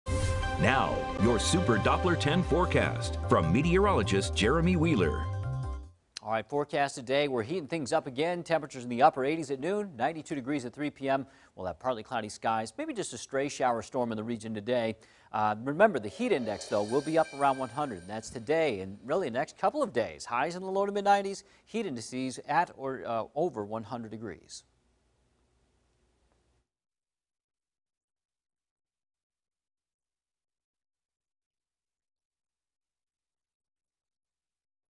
WAVY Weather Morning Update | August 26, 2021
Super Doppler 10 Meteorologist Jeremy Wheeler with the latest forecast for the Hampton Roads region.